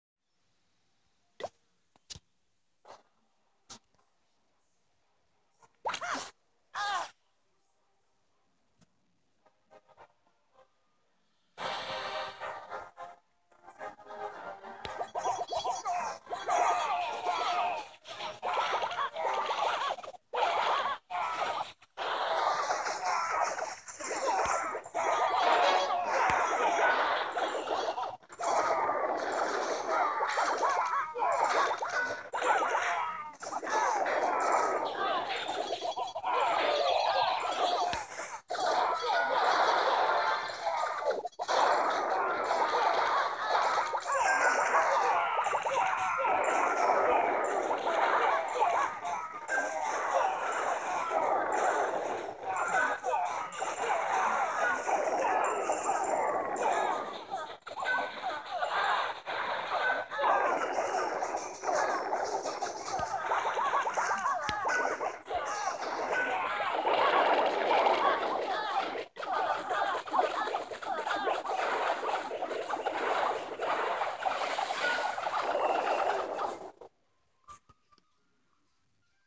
I'm Starting To Like This Strategy...
krazykalvin taking on a TH8 in a raid :)